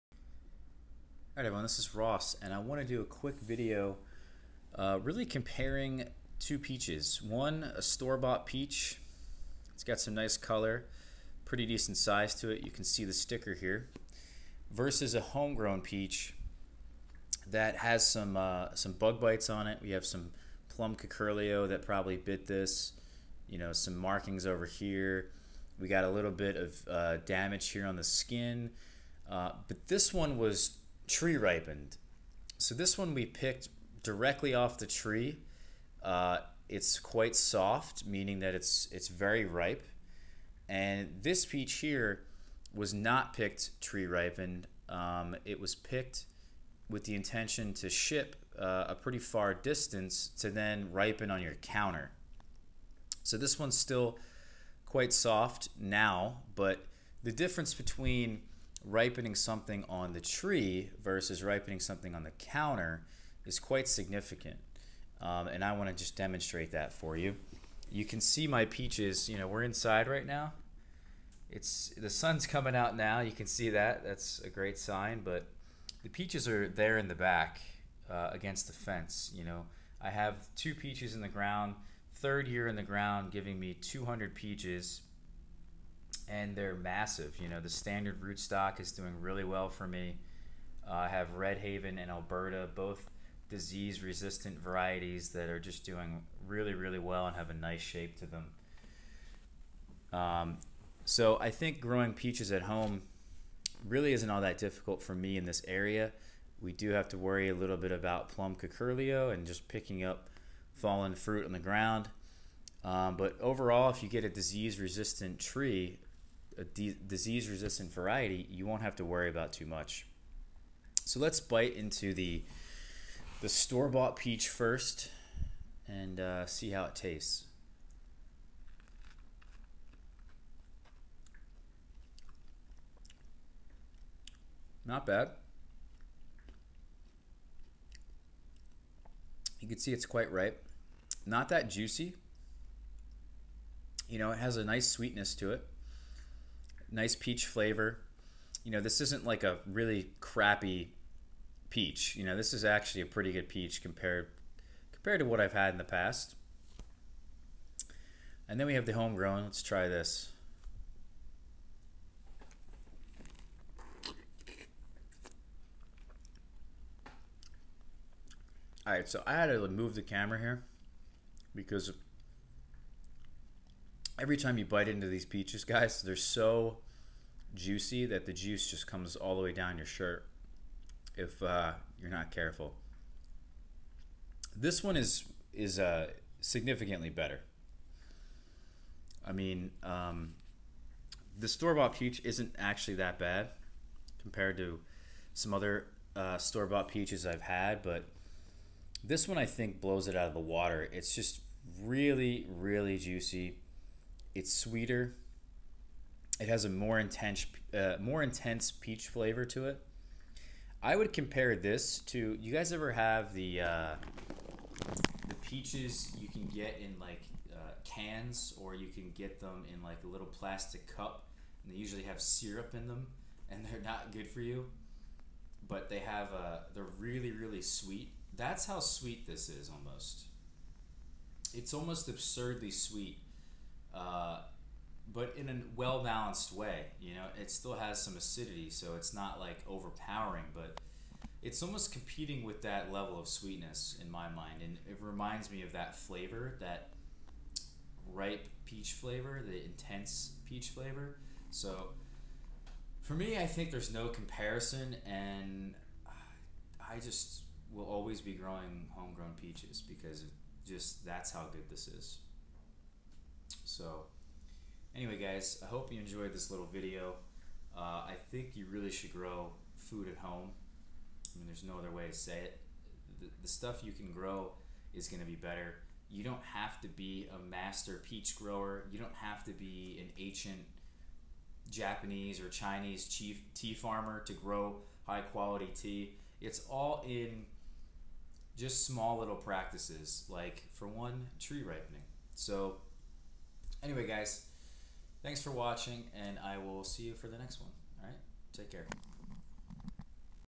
HOME GROWN PEACHES are KING
In this video I get a well ripened home grown peach and compare the flavor to a store bought peach. This is the difference.

Instagram.com/rossraddi
Twitter.com/rossraddi